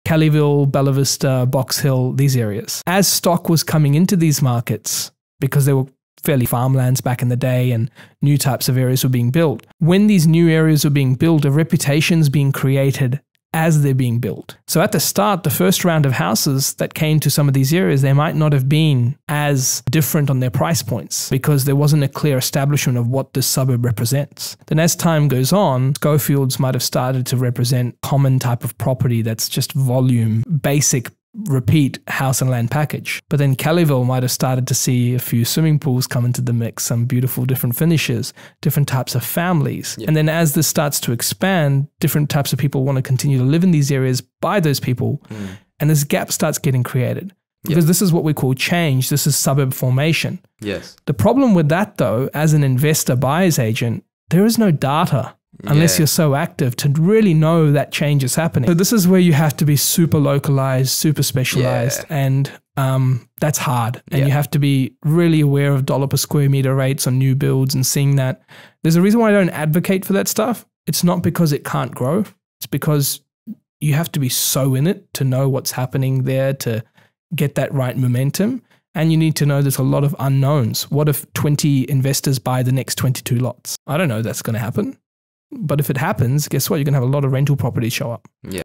Buying into a developing area will need a deep level of knowledge of the locality!
If you are looking into lands that are just starting to get converted into residential properties you will need a massive amount of data and a person with their noses into those properties, people who are not just highly localized but that should be specialized in it. Looking into how lucrative a property is will depend on what data and research you can collect to get a more solid semblance of where your investment may lead you to. But this isn’t always the case for areas that are only just being developed, one area may go from fields to riches, but another may become cookie-cutter replicas of each other.

It's a risk, so you must really understand if this is a risk you are willing to invest in.

Listen to Arjun’s insights on this, at the “Property is a Game of Finance” podcast!